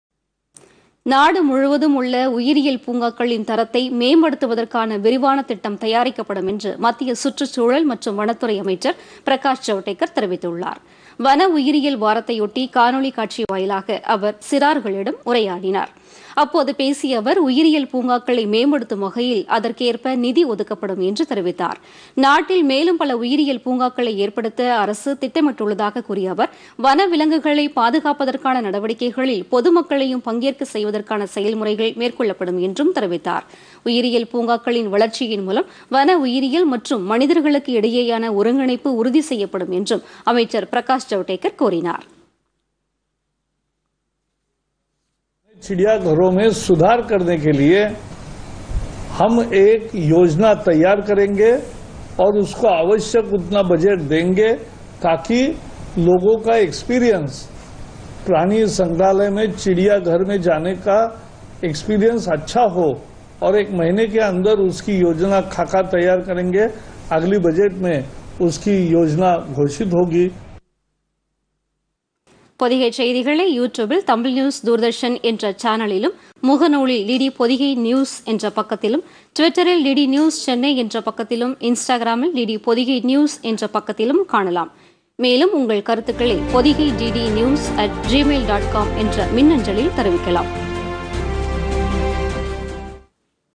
வனவிலங்குகளை பாதுகாக்கும் திட்டம்
வனவிலங்குகளை பாதுகாக்கும் திட்டங்களில் பொதுமக்களும் பங்கேற்க புதிய நடைமுறை செயல்படுத்தப்படும் - மத்திய அமைச்சர் பிரகாஷ் ஜவடேகர்